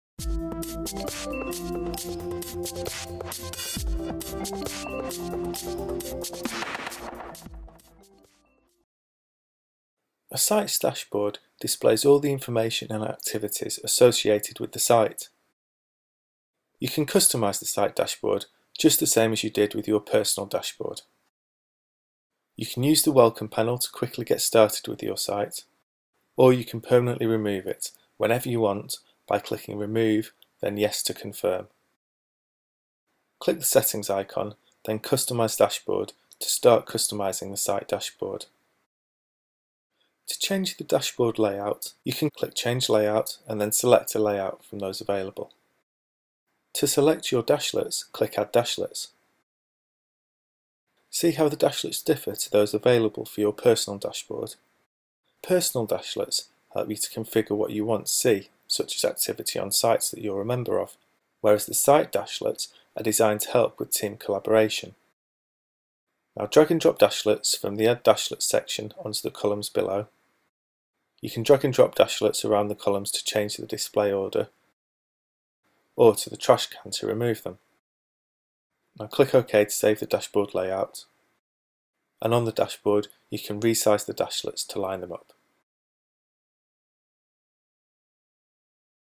Alfresco in the cloud: Customizing a Site Dashboard
- This Alfresco in the cloud video shows you how to customize an Alfresco site dashboard.

Be the first to find out about the latest Alfresco videos and features